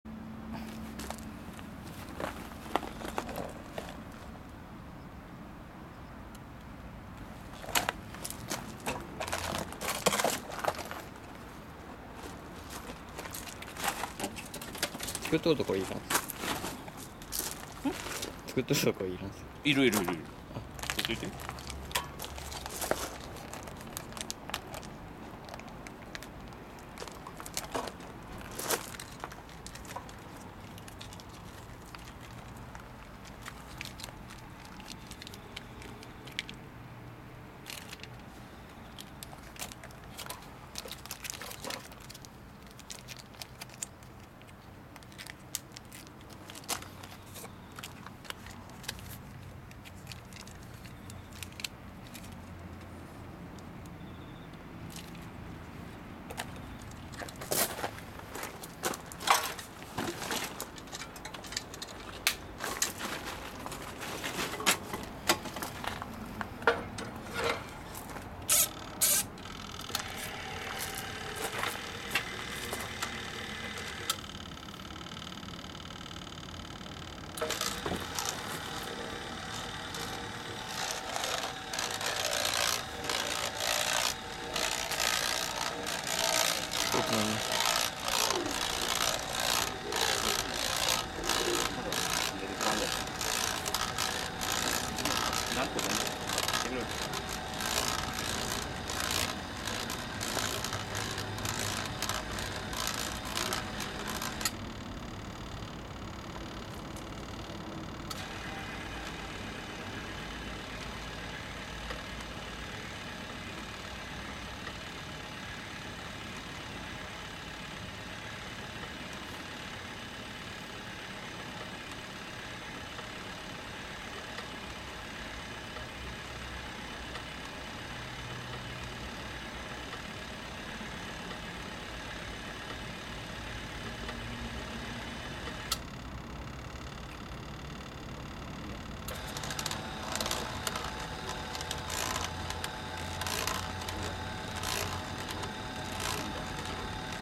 羽島郡 Ｂ様邸 外壁塗装工事（コーキング） 岐阜県＠中村ワークス
外壁塗装工事でコーキング材をかくはん機で混ぜる工程をしております。